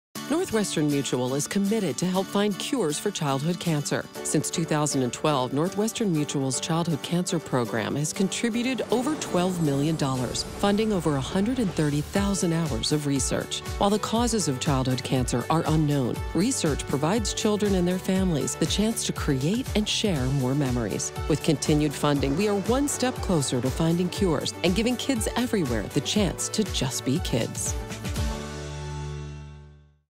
NM supports the search for the Cure
Since 2012, Northwestern Mutual has made a commitment to funding research devoted to childhood cancer. Northwestern Mutual joined the V Foundation and ESPN to shed a spotlight on childhood cancer. A 30 second commercial aired on ESPN in front of the Rose Bowl® Game, featuring our Childhood Cancer Program.